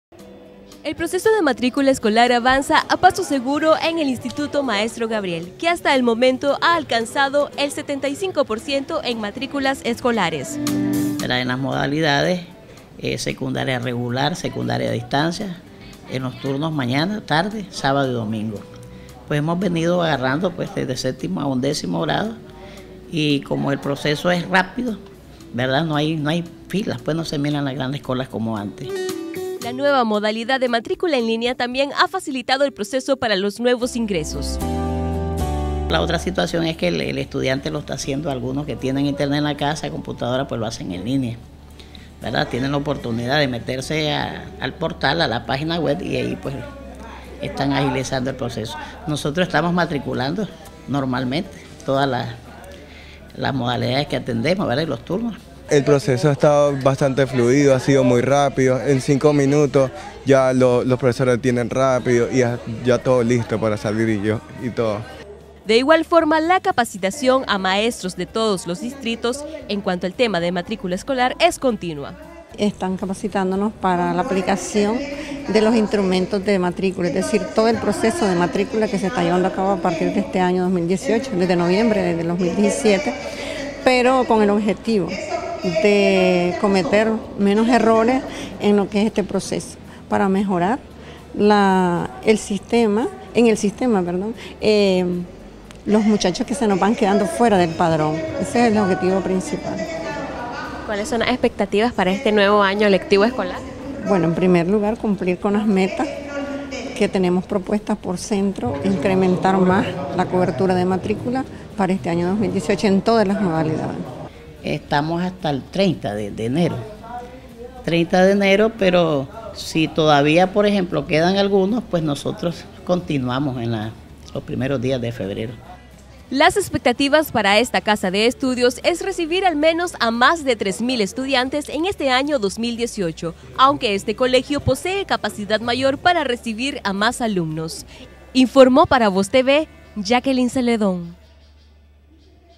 Periodo de matrículas en Nicaragua avanza a paso seguro
En el Instituto Maestro Gabriel de la ciudad de Managua, la matricula avanza en un 75 por ciento.